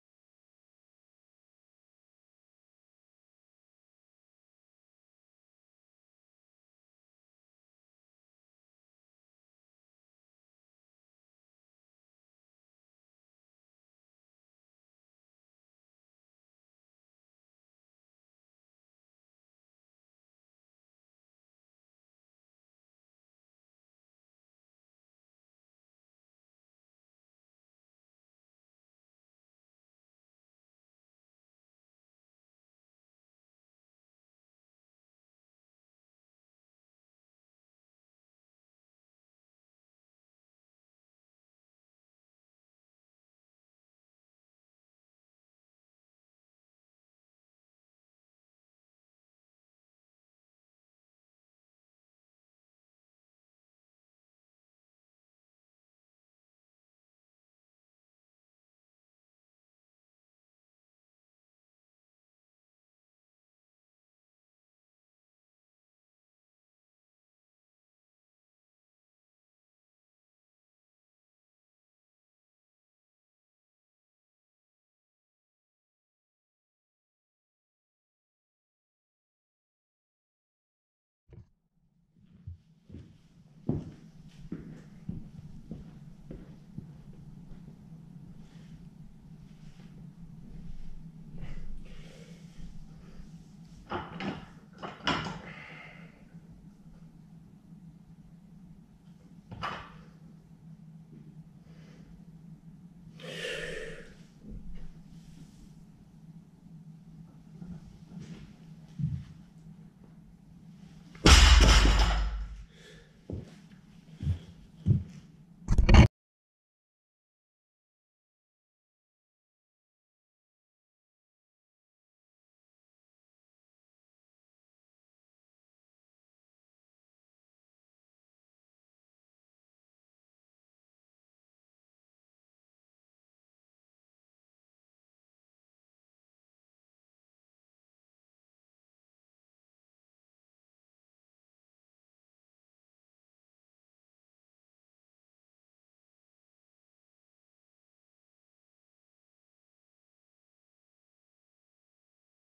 Bulgarian Training 23/5-19
Today I couldn't overhead press heavy because of the cramping, but who cares. I'm not gonna take a random deload for a tiny little cramping muscle tweak.